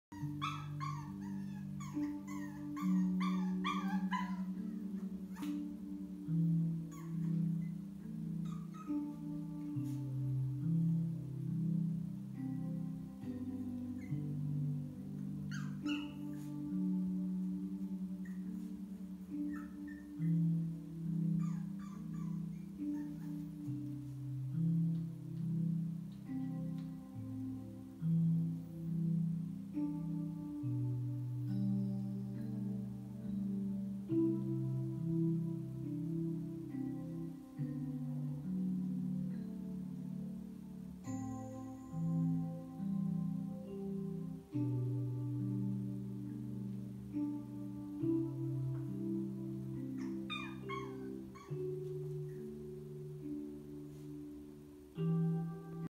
[심쿵포미] Don’t worry baby, no separation anxiety plz, a poor pomeranian puppy
[심쿵포미] 낑낑 맴찢 분리불안증은 안되는데, pomeranian puppy

귀여운 포메라니안 강아지 일상